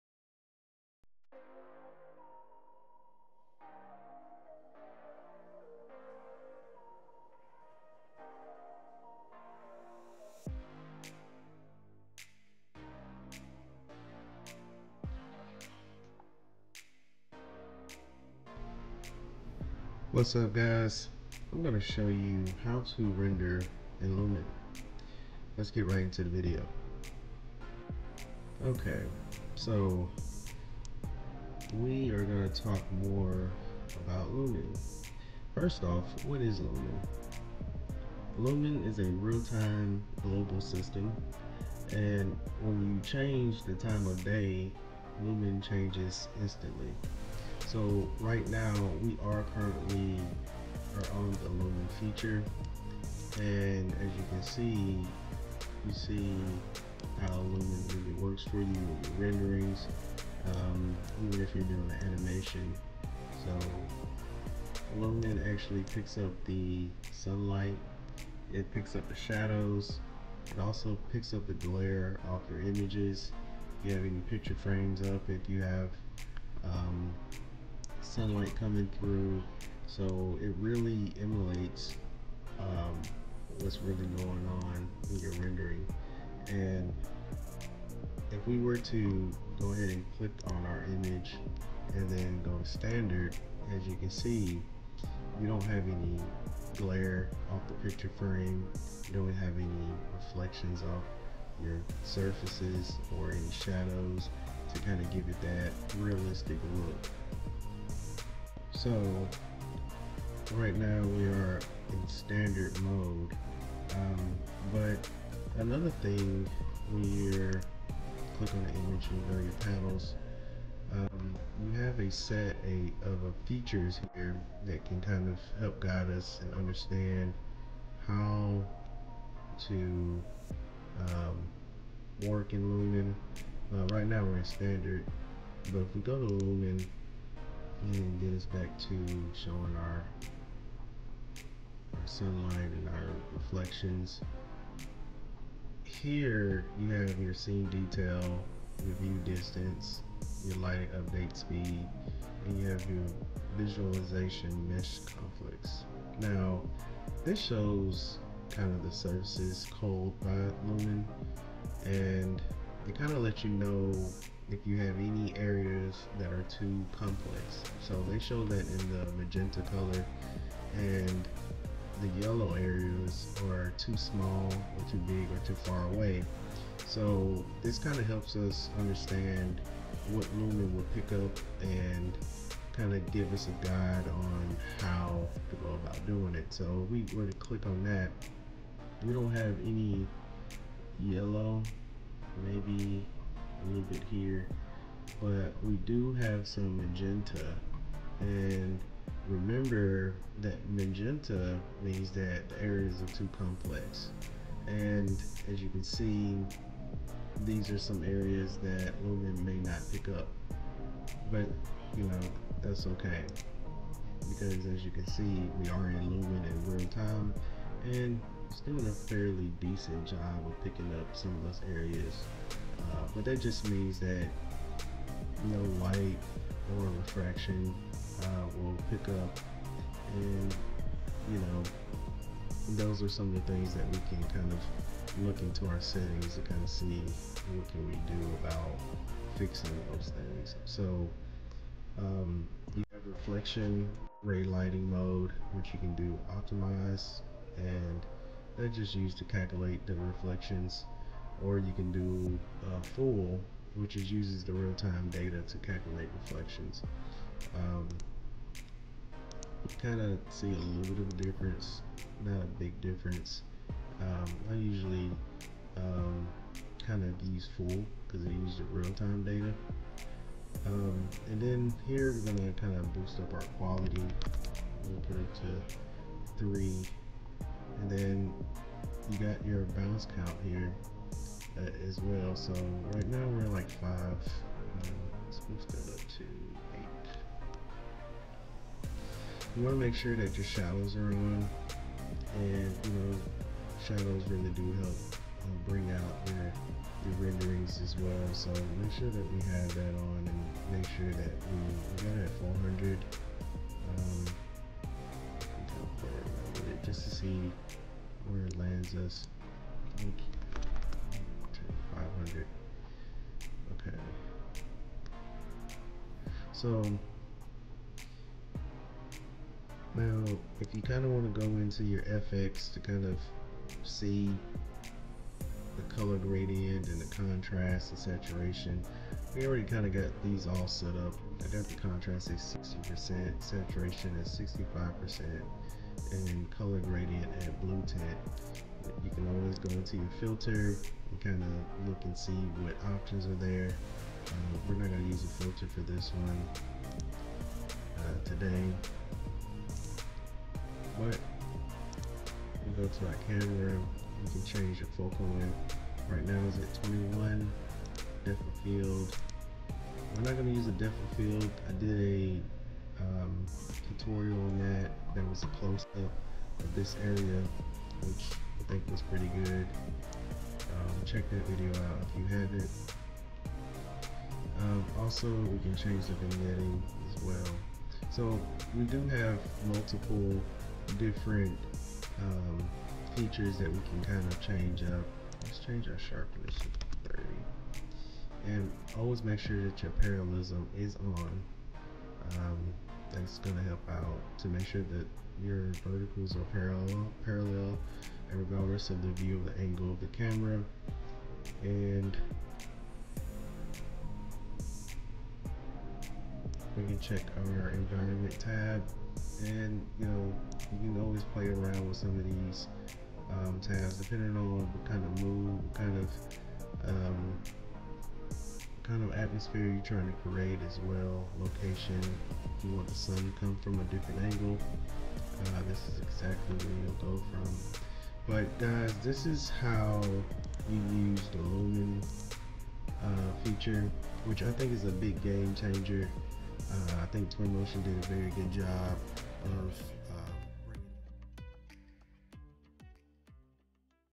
Twinmotion 2023.2 Tutorial: Real-time Rendering With Lumen
🔥👇🏽Struggling with Revit + Twinmotion? 6 Hacks for Faster, More Realistic Renders!

In this video you will learn:
How to use LUMEN in twinmotion 2023.2. I’ll share my tips on what I use to help create real-time interior renderings using lumen. Lumen is based on raytracing, a powerful rendering technique that generates realistic lighting, reflections, shadow effects, and dynamic global illumination features with real-time performance in Twinmotion. By the end of this video, you’ll have the tools to help you unlock your true potential using Twinmotion.

Look out for more updates for more videos on real-time rendering, and ArchViz.

This is done using twinmotion by Epic games. This powerful rendering tool helps you quickly create high quality Architecture visualizations and inspires new ways to enhance your skill set in real-time rendering.